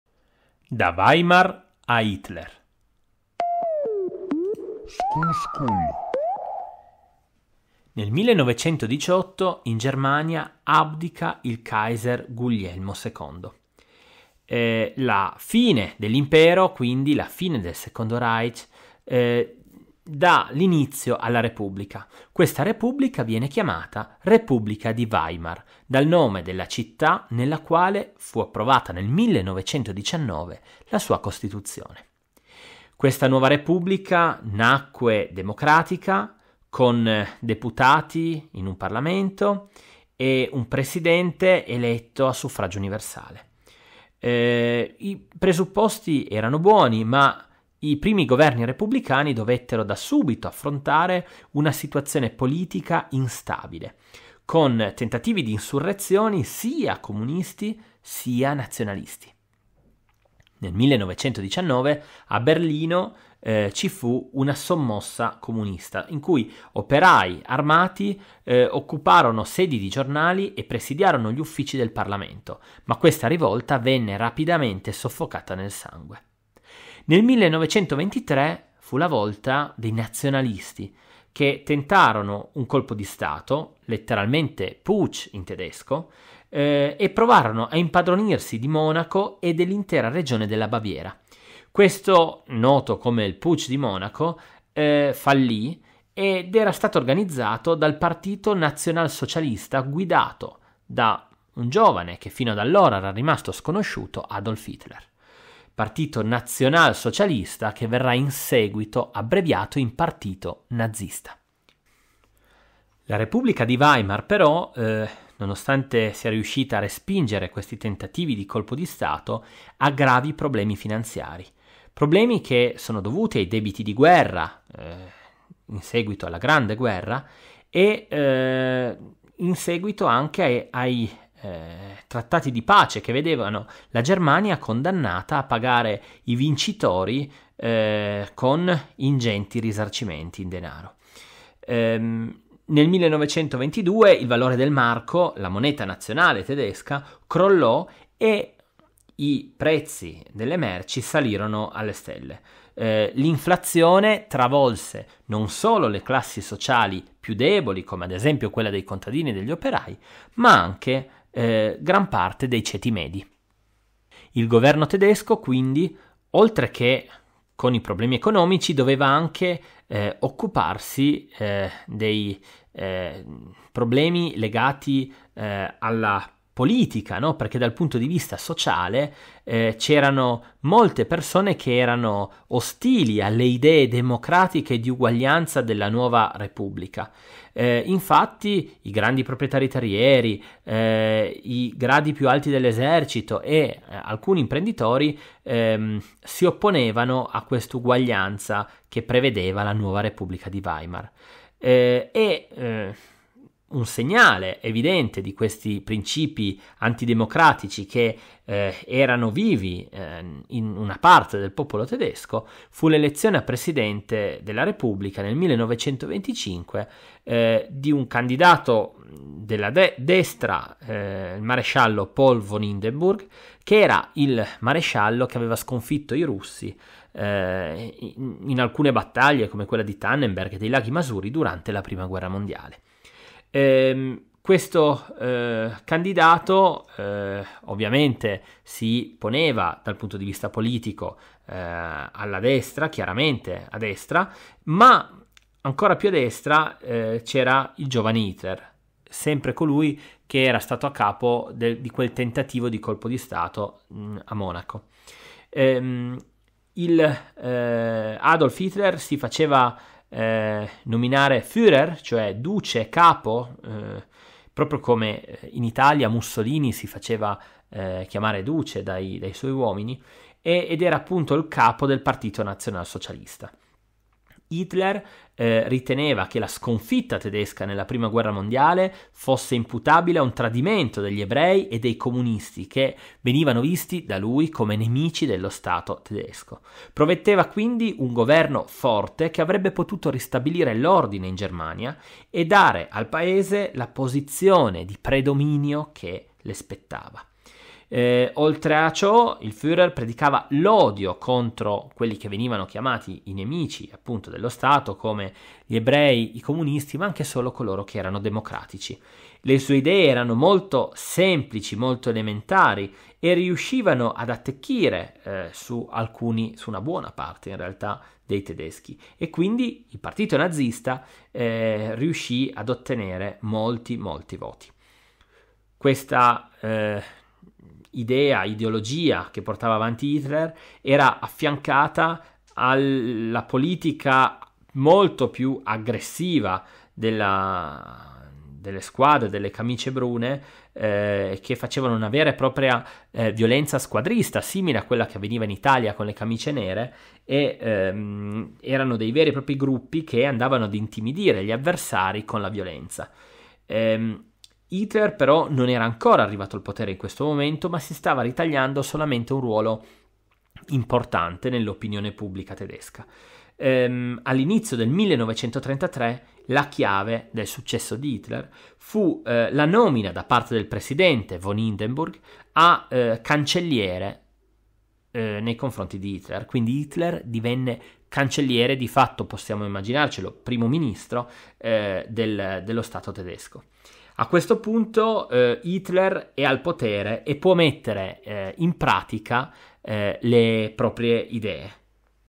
La Germania da Weimar a Hitler
In questo video parleremo della Germania dalla Repubblica di Weimar all'ascesa di Adolf Hitler.

Se vuoi approfondire questo argomento:
"La Germania di Weimar" di Weitz

"Morte della democrazia" di Carter Hett

"Breve storia del nazismo" di Corni